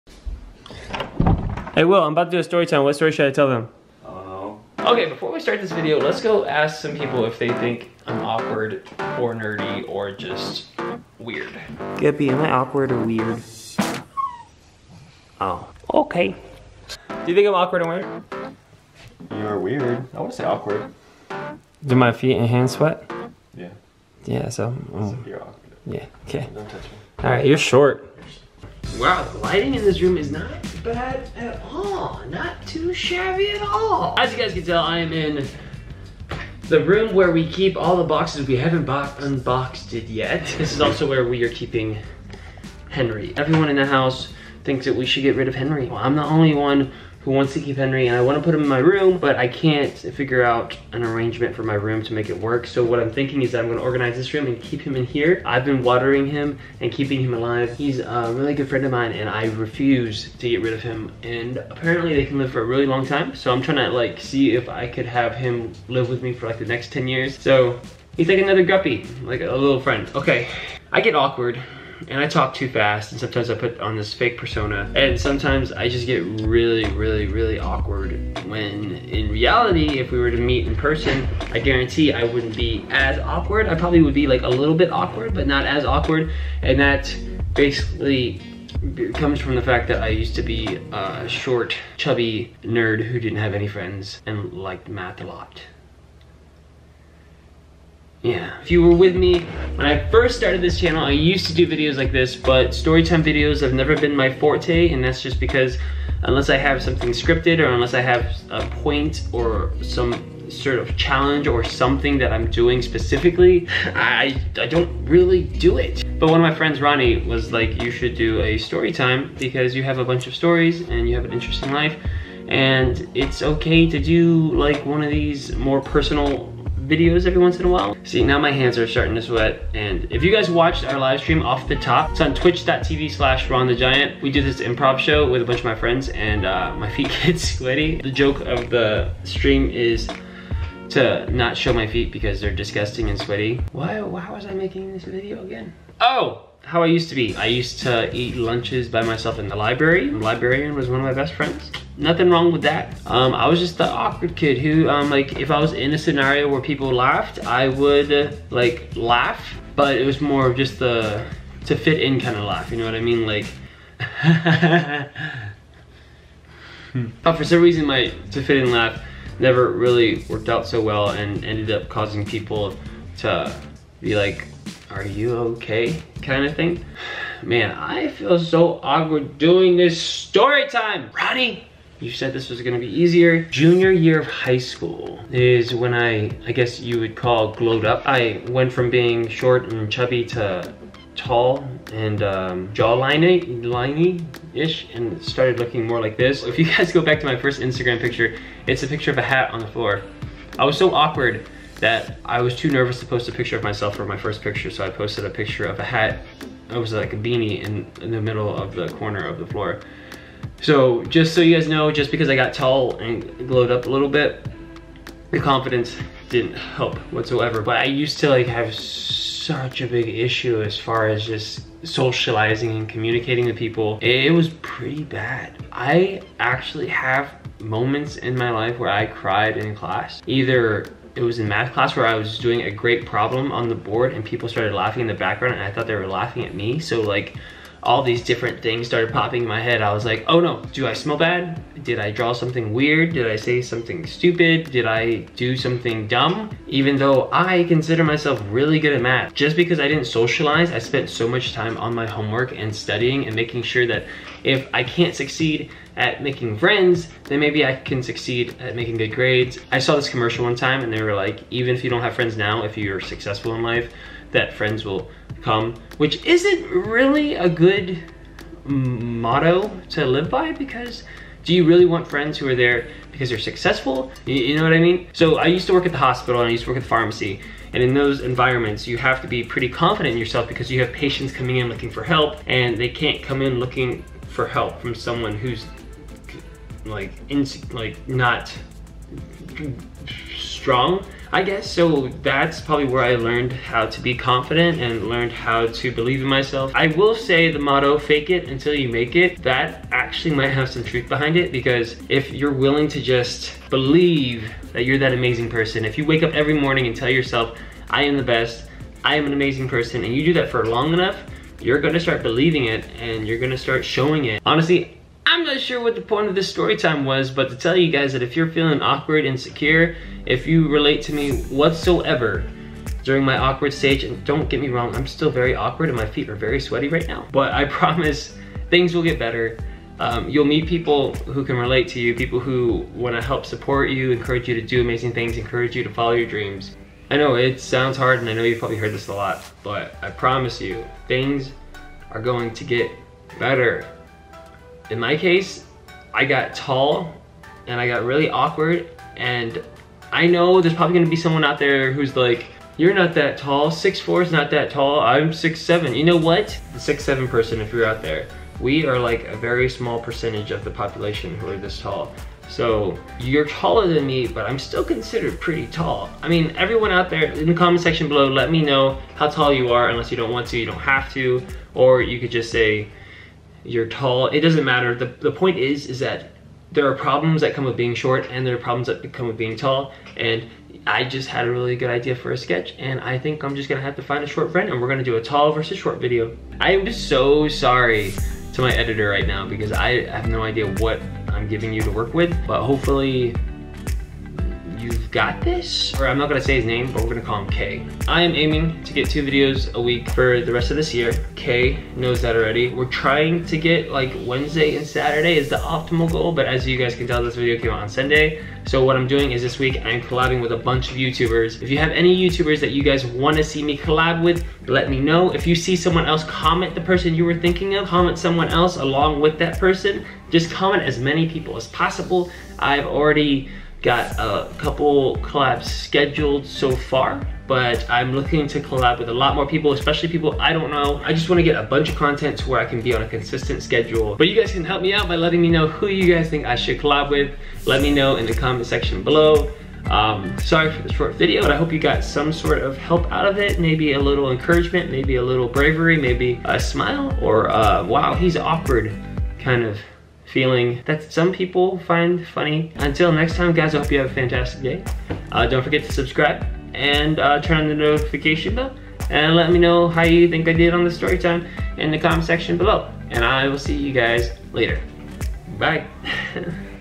I Cried In Front Of My Whole Class!! (Bullied)
Opening up with a storytime about the time I cried in front of my entire class and how I was very unpopular and awkward.